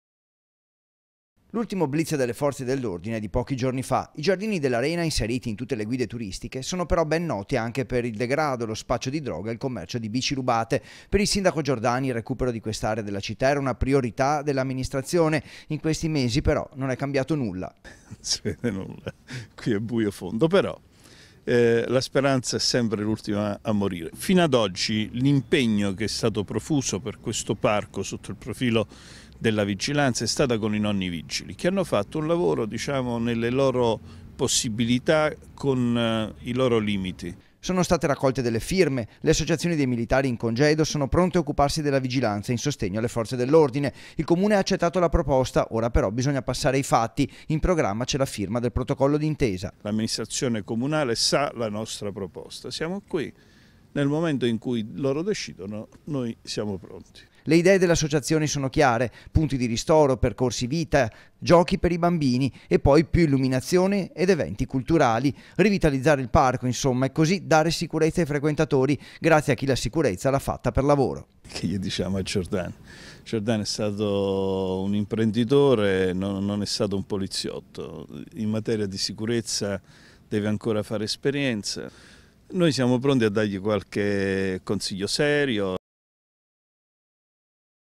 TG (13/02/2018) - GIARDINI ARENA: VIGILANZA AI VOLONTARI, SERVE LA FIRMA DEL COMUNE
TG (martedì 13 febbraio 2018) - L’ultimo blitz delle forze dell’ordine è di pochi giorni fa. I Giardini dell’Arena, inseriti in tutte le guide turistiche, sono però ben noti anche per il degrado, lo spaccio di droga e il commercio di bici rubate. Per il sindaco Giordani il recupero di quest’area della città era una priorità dell’amministrazione, in questi mesi però non è cambiato nulla.Sono state raccolte delle firme, le associazioni di militari in congedo sono pronte ad occuparsi della vigilanza in sostegno alle forze dell’ordine. Il comune ha accettato la proposta, ora però bisogna passare ai fatti, in programma c’è la firma del protocollo d’intesa.le idee delle associazioni sono chiare. Punti di ristoro, percorsi vita, aree per i bambini. E poi più illuminazione e eventi culturali. Rivitalizzare il parco, insomma, e così dare più sicurezza ai frequentatori grazie a chi la sicurezza l’ha fatta per lavoro.